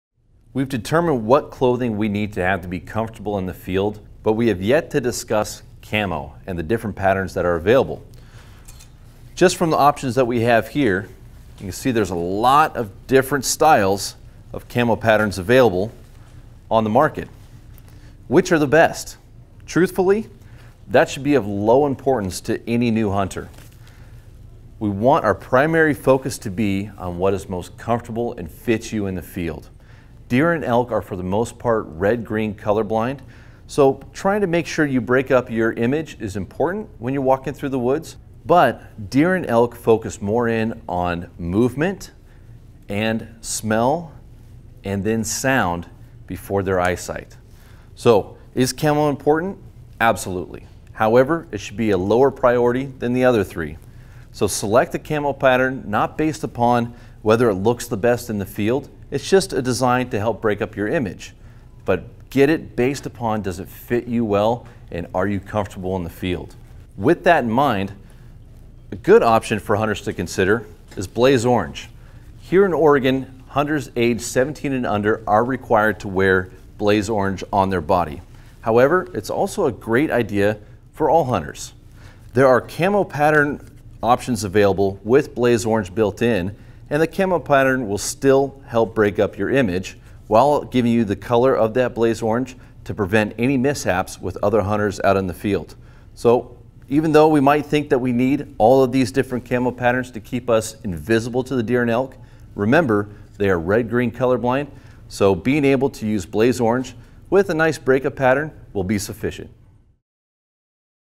How to hunt for deer & elk in Oregon - Camo Segment
This video discusses the types of camo used when hunting deer & elk in Oregon.  The Oregon Department of Fish and Wildlife offers classes in how to hunt for deer and elk.  Visit MyODFW.com for more information about classes, fish, wildlife and conservation.